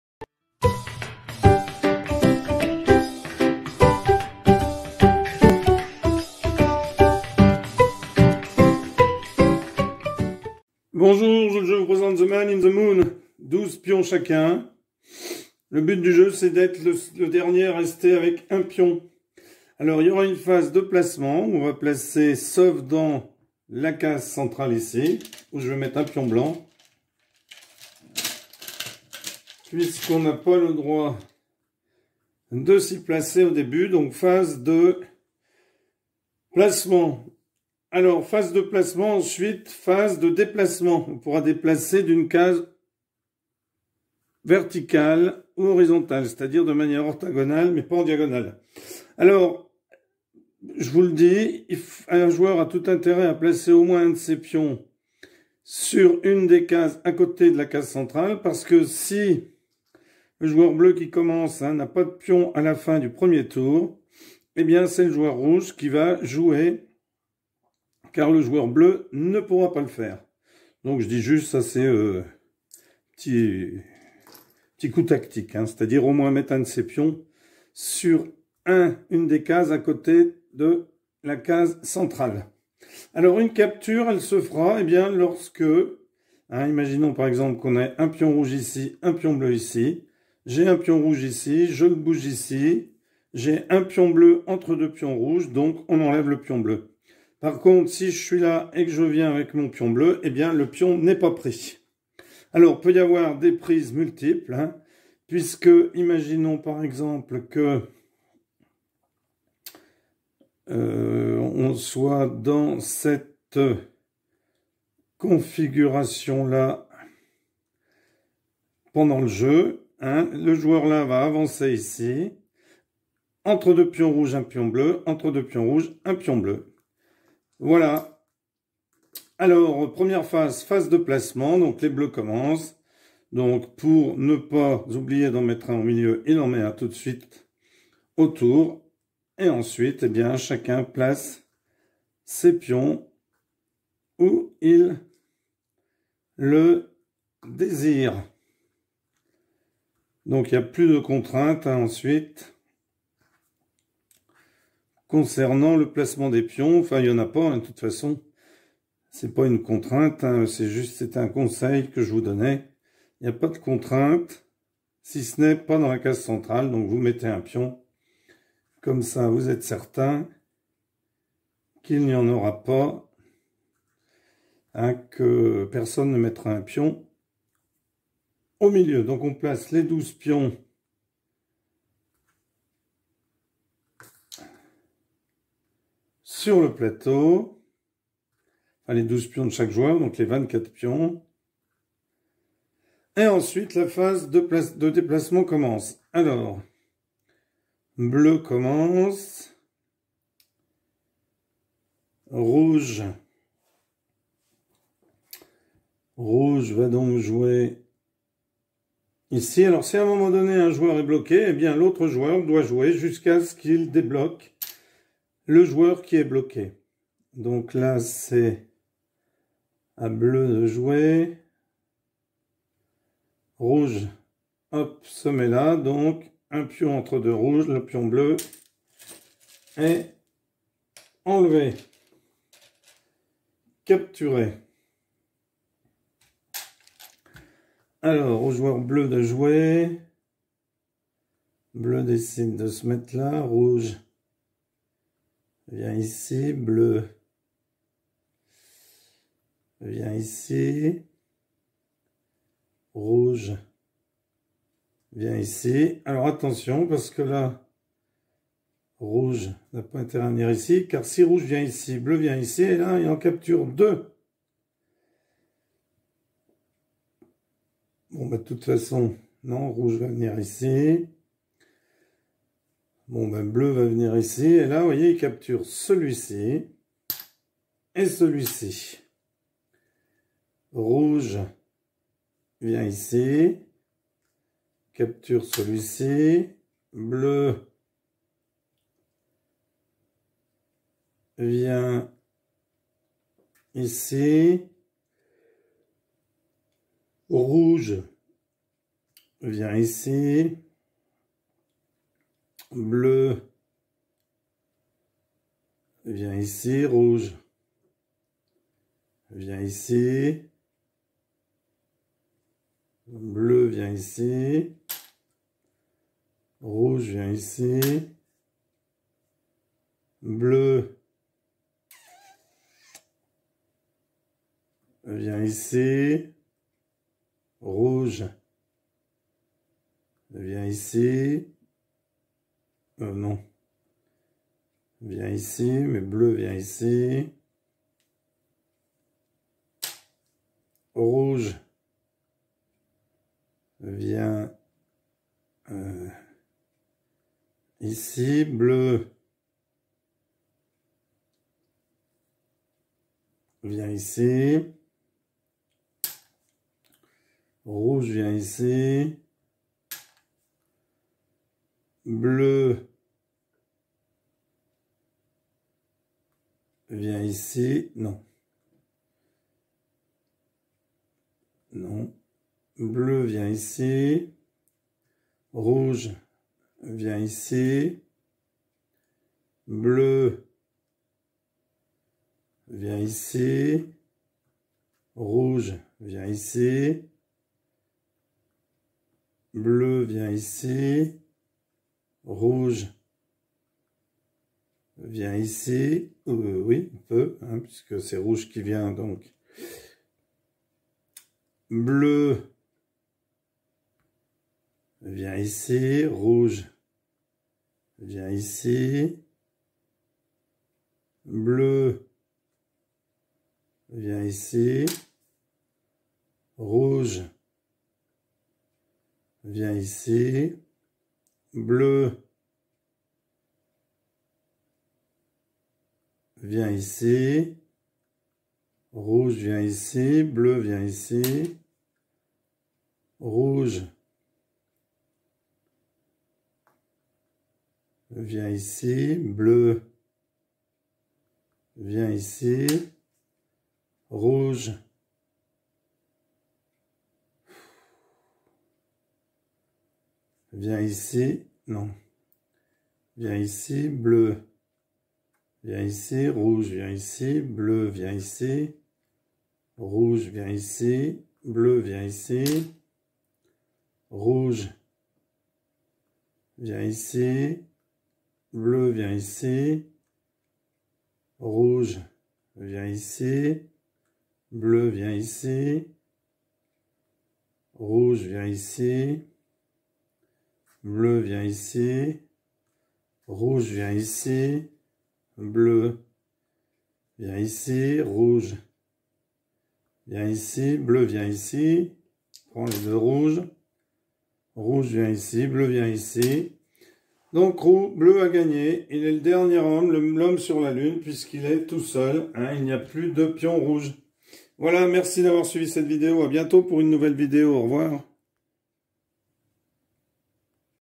Règle du jeu The man in the moon
Un plateau de vingt-cinq carrés et deux séries de douze pions aux couleurs contrastées composent L'Homme dans la Lune .
Le but du jeu est d'éviter de devenir "l'Homme dans la Lune".
Le premier joueur place un de ses pions sur n'importe quelle case du plateau, à l'exception de la case centrale. Ensuite, le deuxième joueur place un de ses pions sur n'importe quelle case du plateau, à l'exception de la case centrale, et ainsi de suite jusqu'à ce que les vingt-quatre pions aient été placés, laissant la case centrale vide.
Le premier joueur déplace maintenant un de ses pions vers la case centrale. Le deuxième joueur joue alors à son tour et chacun joue successivement. Les pions peuvent être déplacés orthogonalement, mais pas en diagonale.
Lorsqu'un joueur flanque un ou plusieurs pions adverses entre deux des siens, ils sont tous capturés tant qu'il n'y a pas d'espace vide intermédiaire. (Similaire à la conversion de flanc d' Othello .)
Les pions ne peuvent être capturés que par un joueur en mouvement. Si un ou plusieurs pions sont pris entre deux pions adverses alors que les pions sont placés sur le plateau, cela ne compte pas comme une capture. La capture ne commence que lorsque les vingt-quatre pions ont été placés sur le plateau.
Si un joueur ne peut pas bouger parce que ses pions sont bloqués, l'autre joueur doit continuer à bouger jusqu'à ce que le blocus soit levé. Ensuite, le joueur bloqué prend son tour et le jeu continue comme avant. Cette règle s'applique également au premier coup du jeu.
Le joueur qui réussit à réduire son adversaire à un pion remporte la partie, le seul pion restant sur le plateau devenant "l'Homme dans la Lune".